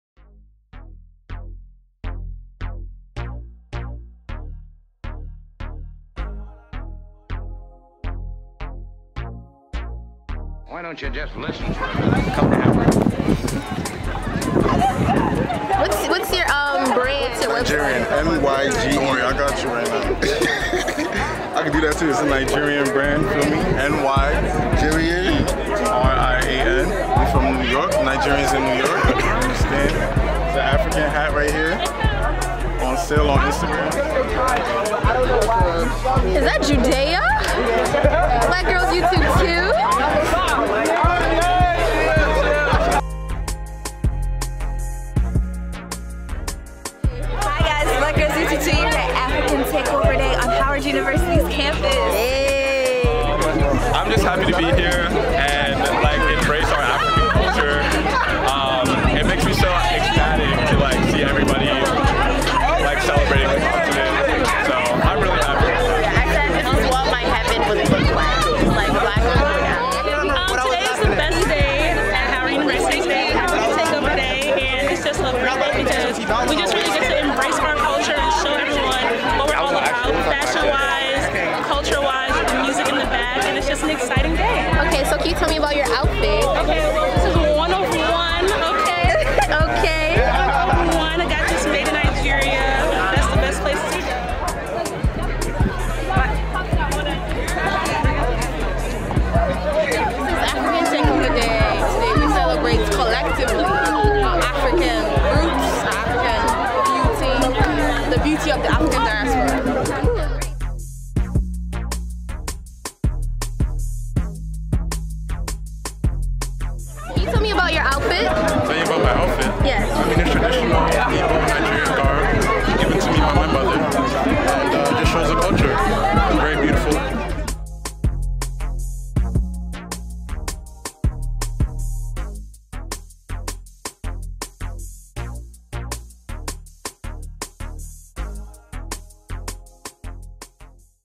HU Take Over Africa Day
I just wanted to show you a quick snippet of what Take Over Africa Day looked like at Howard University.